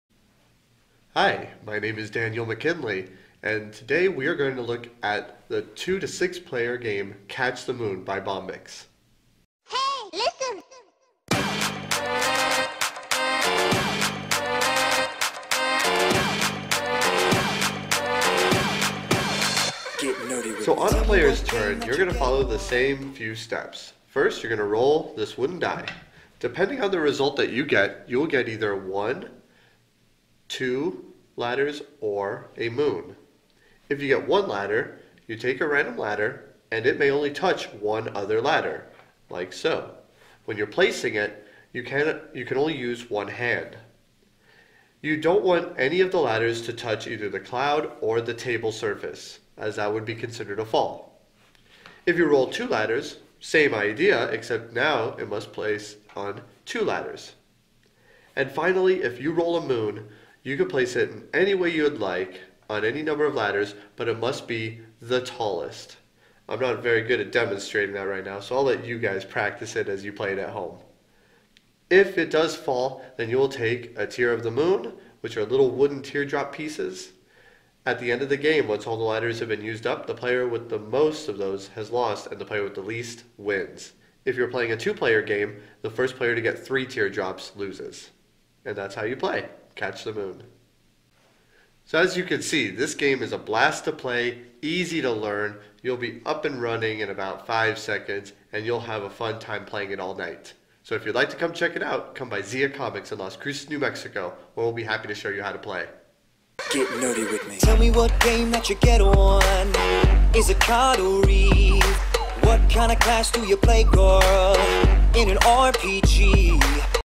How to Play Catch the Moon by Bombyx
Danny from ZiaComics.com teaches us how to play Catch the Moon by Bombyx.  This is a dexterity puzzle game for 2-6 players and should take 20 minutes for a full game.  The rules are simple and you can be up and running in minutes.

All you need in Catch the Moon is a few skillfully placed ladders, a good sense of balance, and a touch of imagination. The moon waits impatiently for your arrival, but she's a sensitive lady and the smallest mistake can make her cry. The right mix of skill and luck will help you become the most agile dreamer...